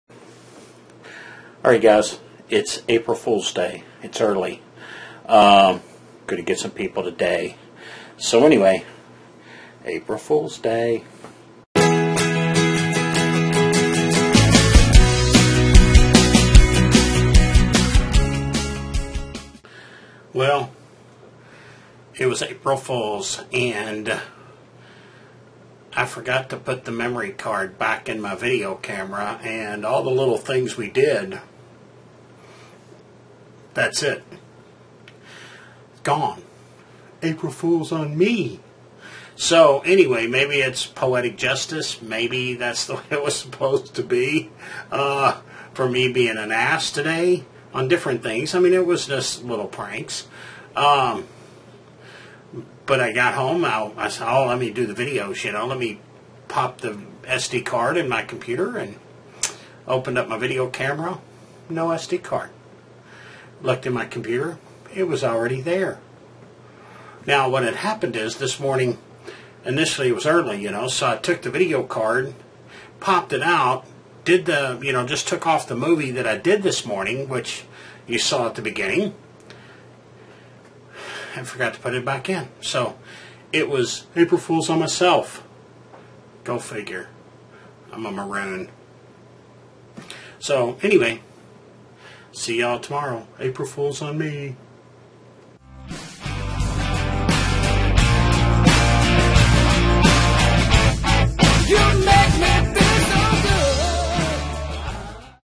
April Fools on Me...
Dang it, I forgot to put my memory card back into the camera and missed all the april fools today